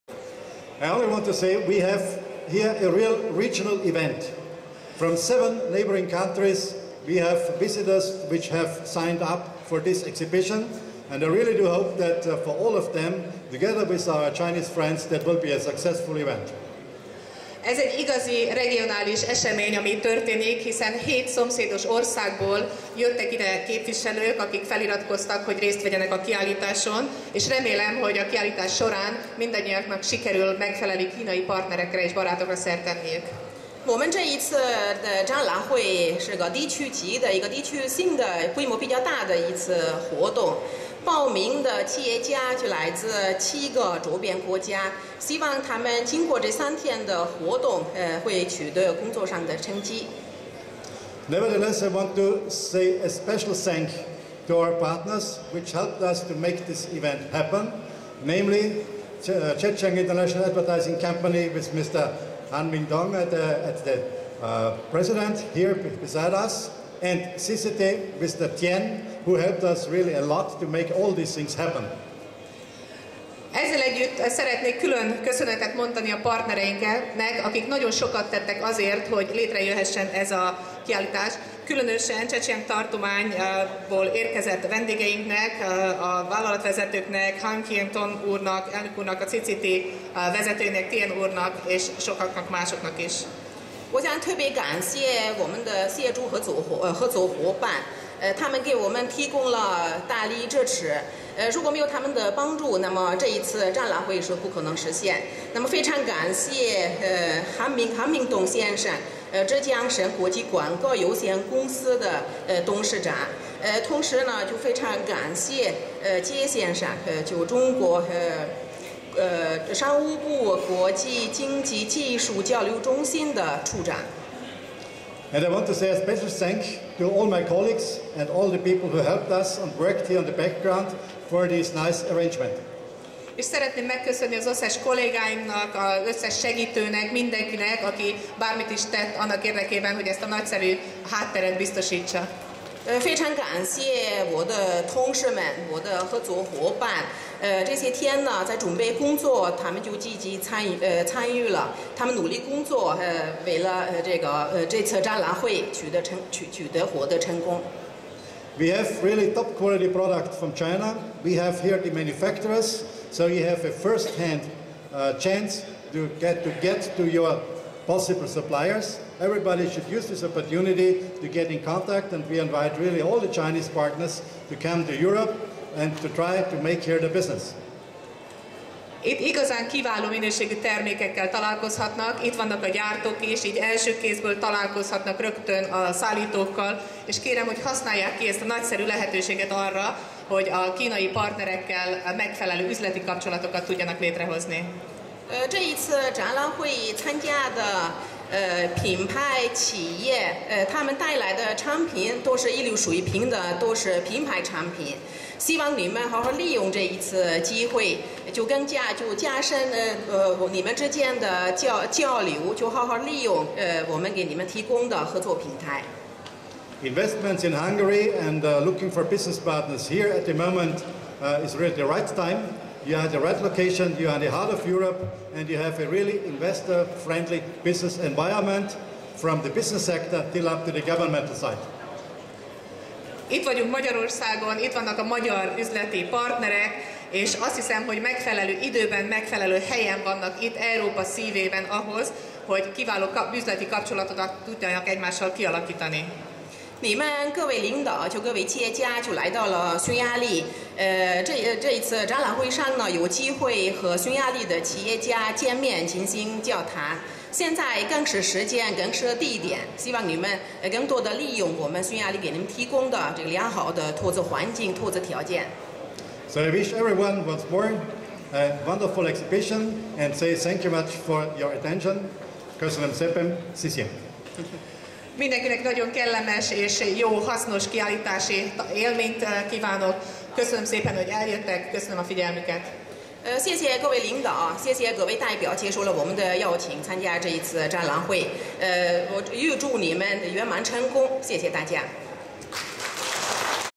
Rudolf Riedl - China Brands Exhibition-3. 2012.05.23. Beszéde 2/1
Rudolf Riedl az AsiaCenter Ügyvezetője

3. Kínai Márkák Kiállítása